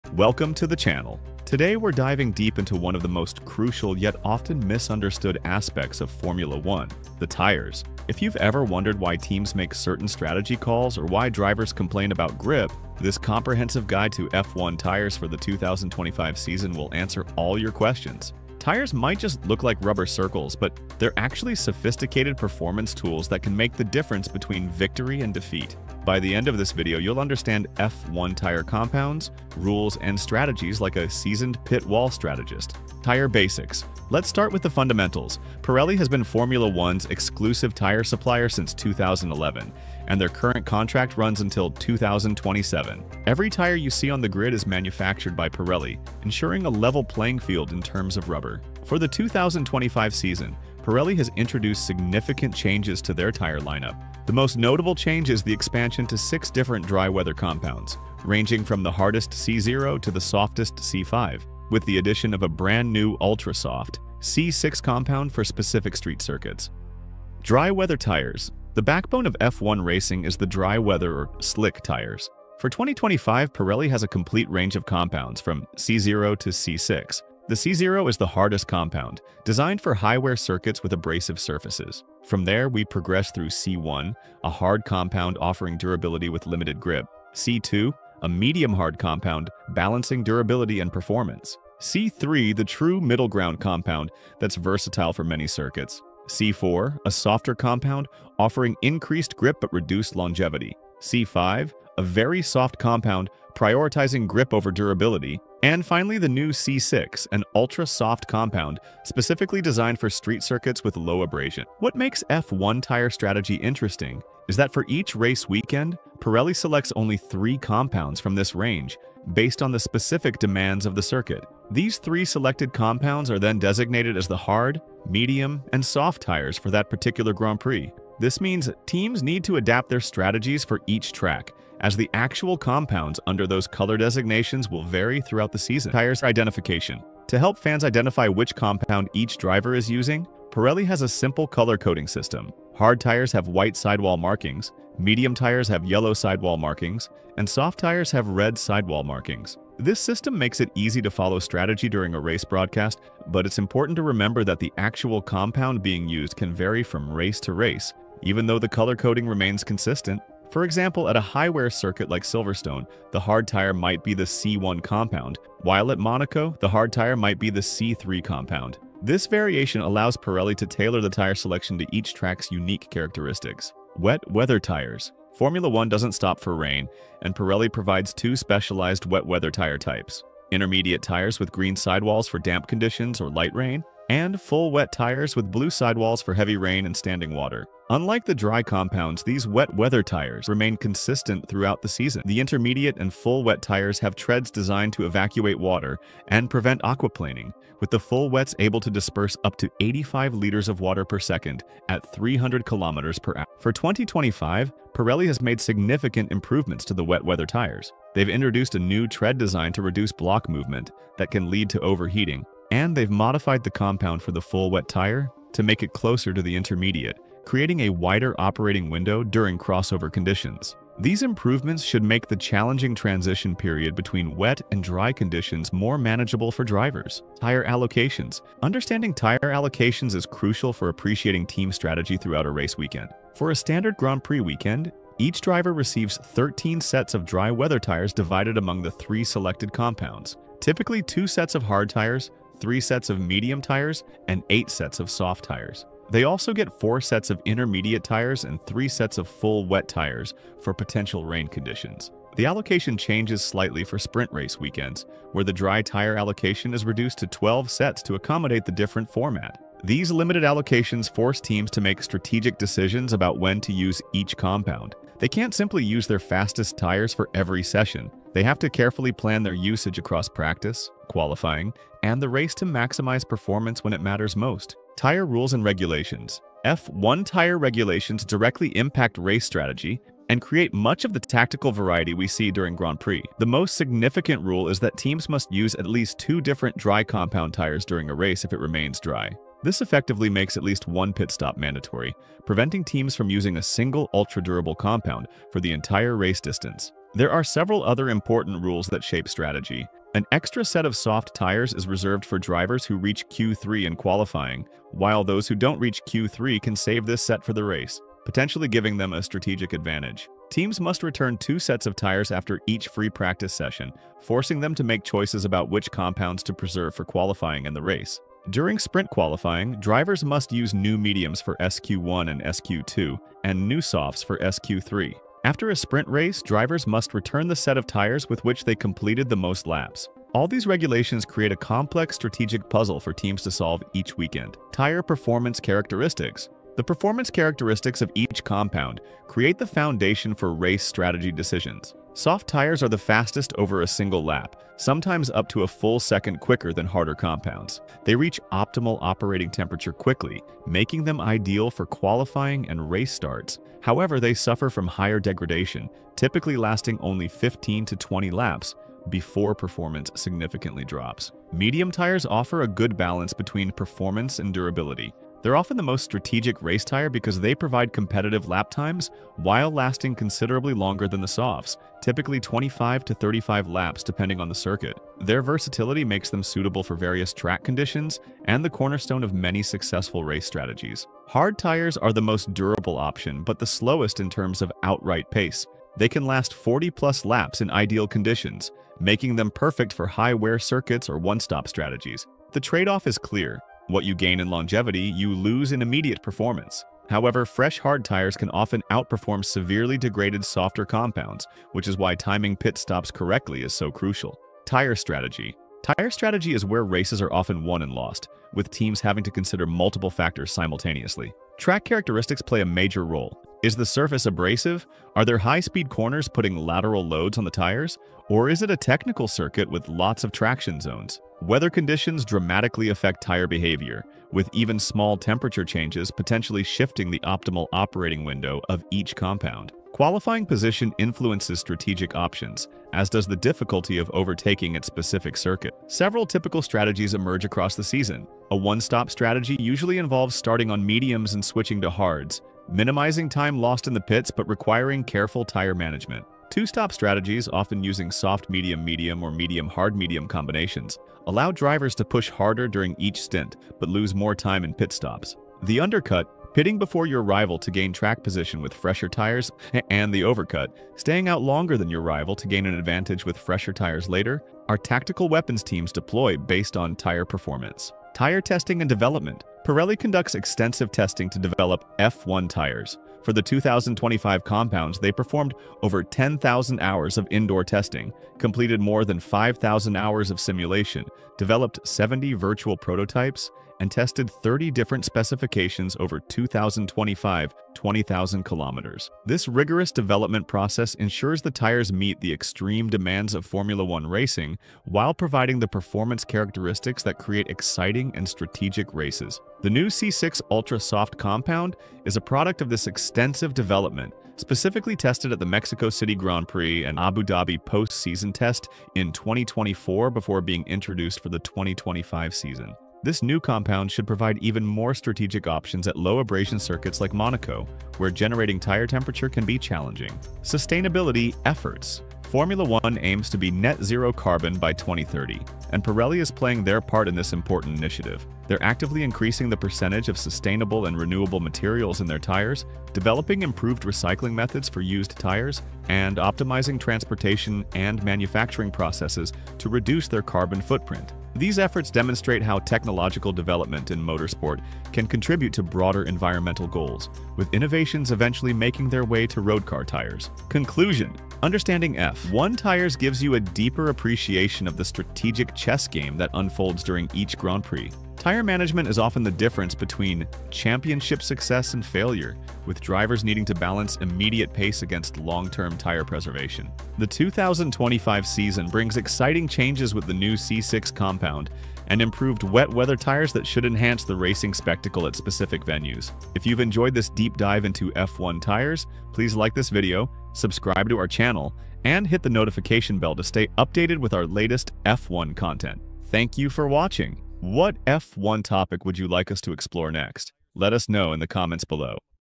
BLACK GOLD: F1's Secret Weapon (2025 Tire Masterclass)
Unlock the mysteries of Formula 1's most critical performance component - the tires. In this comprehensive guide to the 2025 F1 tire regulations, we explore Pirelli's expanded range including the brand new C6 ultra-soft compound, the science behind tire strategy, and how the right rubber choices make the difference between victory and defeat.
This video breaks down:
All 7 dry compounds (C0-C6) and their unique performance characteristics
Improved wet weather tire technology for 2025
The complex rules governing tire allocation and usage
The strategic chess game of tire management during race weekends
Pirelli's sustainability initiatives as F1 moves toward carbon neutrality
Whether you're new to F1 or a seasoned fan, this masterclass will transform how you watch races, helping you understand pit wall decisions and driver complaints about grip. Perfect viewing before the next Grand Prix!